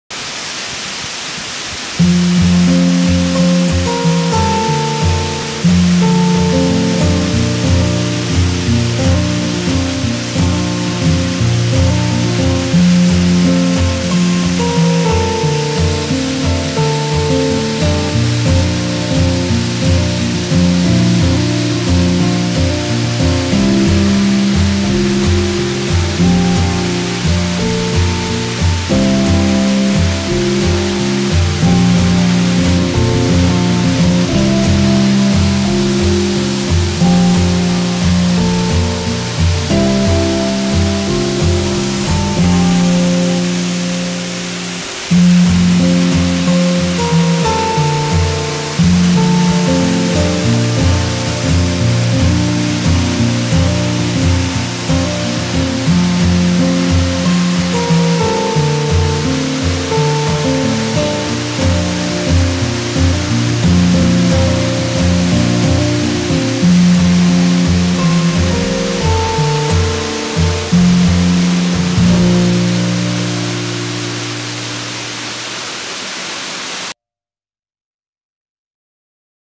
Natural Habitat of Bucephalandra sp Melawi Near a Waterfall
This Bucephalandra grows on some rocks near a waterfall. High humidity is an absolute requirement for Buce to survive and grow well. The location of this waterfall is in Serawai Subdistrict, Melawi.